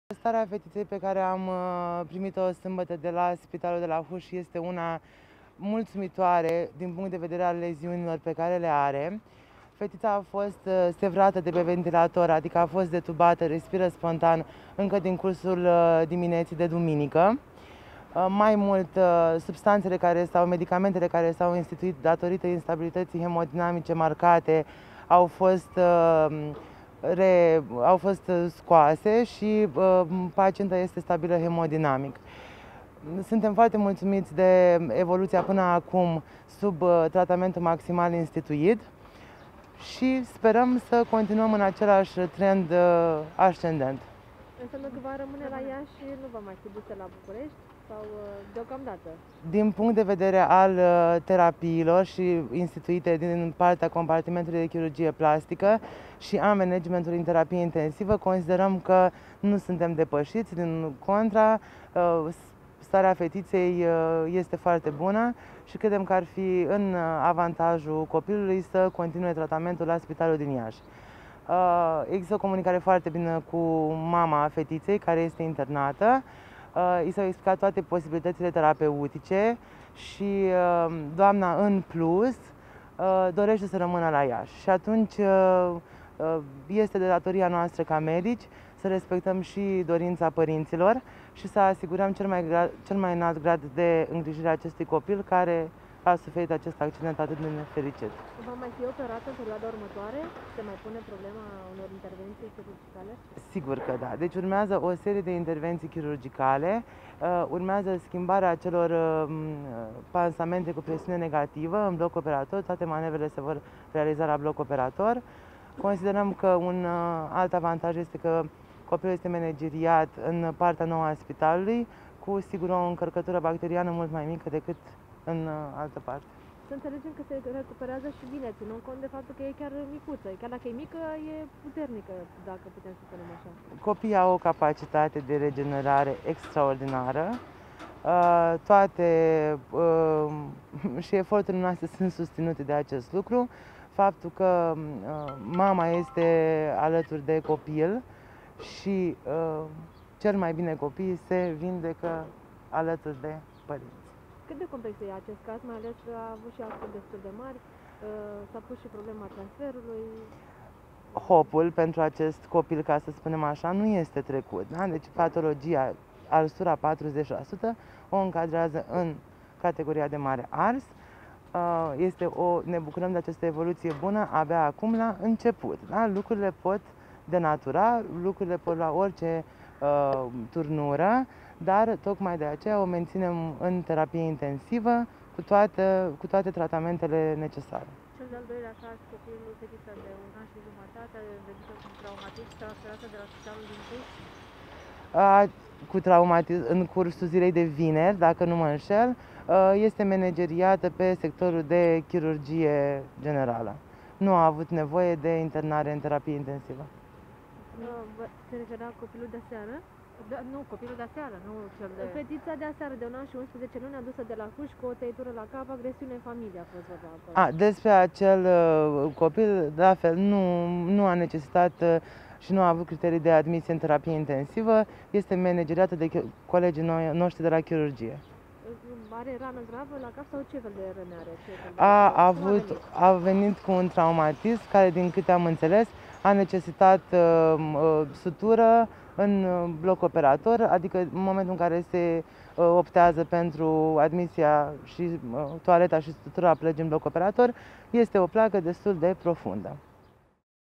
Care este starea fetiţei din Vaslui care a fost opărită cu ciorbă şi transportată la Iaşi
Lavinia Ionescu, purtător de cuvânt al Spitalului Sf. Maria a transmis astăzi, 2 martie 2021, faptul că starea de sănătate a fetiţei care a fost opărita cu ciorba şi transportată la Iaşi este una bună. Copila a fost detubată şi în aceste momente respiră spontan.